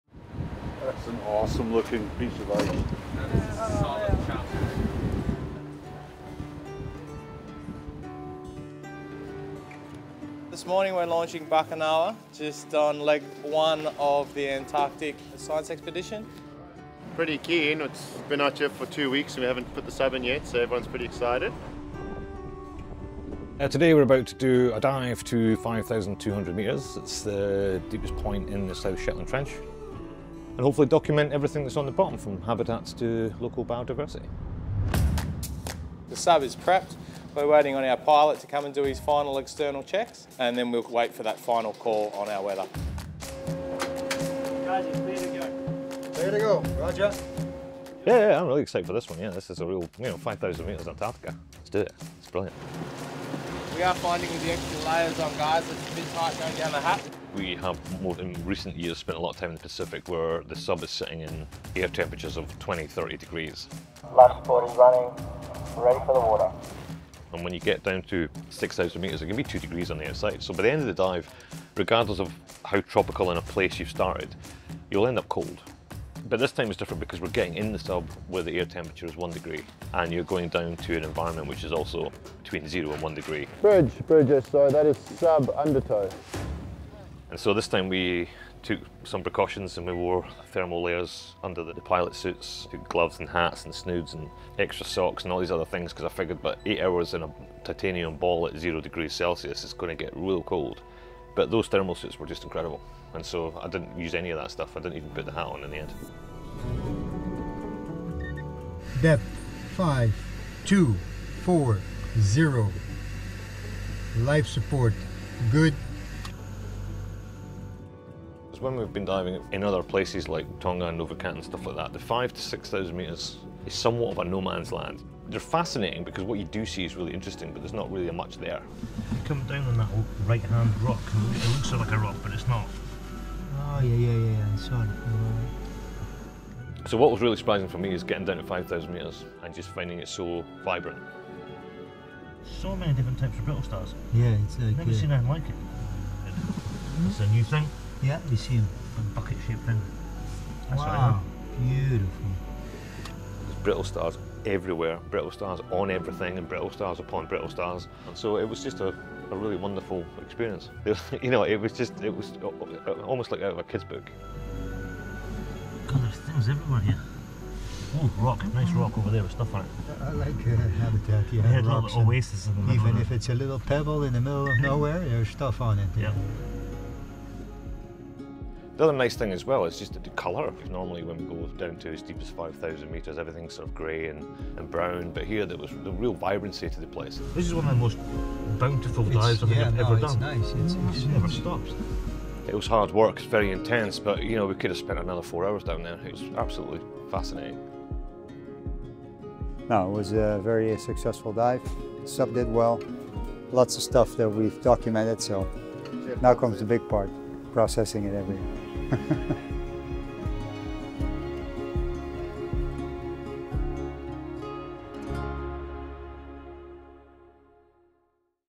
Diving 5000 Metres Deep Beneath Antarctica
A colourful oasis in the middle of nowhere..

Diving deep in our submersible Bakunawa, we explore and map the habitats and local biodiversity at 5,200 metres deep in Antarctica!

‘It’s like something out of a children’s book ‘ - Professor Alan Jamieson

Thanks to our partners @deepseauwa